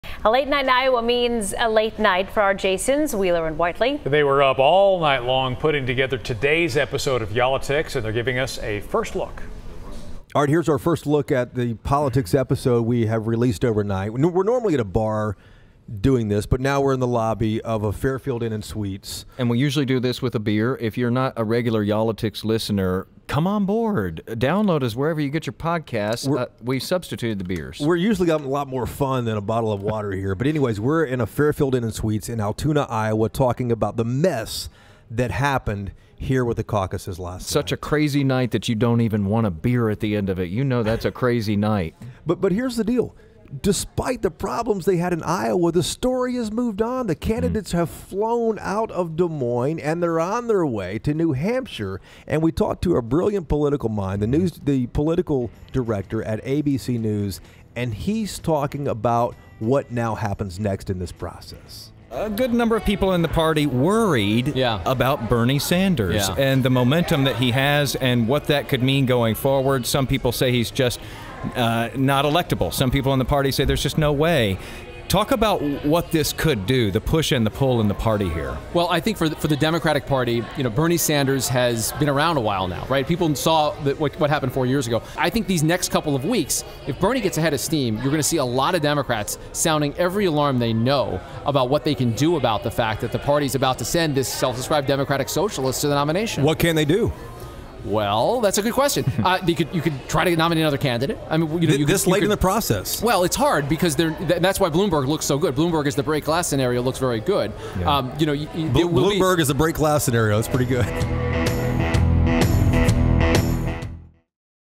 Does it matter now who won in Iowa?
Election 2020 is already off to a wild start. The Iowa Democratic Party couldn’t report an accurate count of vote totals on caucus night, raising lots of questions.